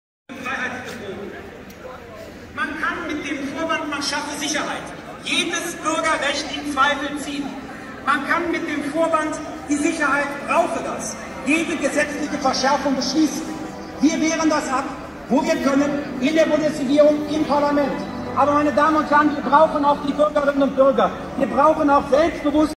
Demo für Frieden, Freiheit und ein friedliches selbstbestimmtes Leben am 07.08.2023 in Bamberg
Eintreffen zur Kundgebung am Gabelmann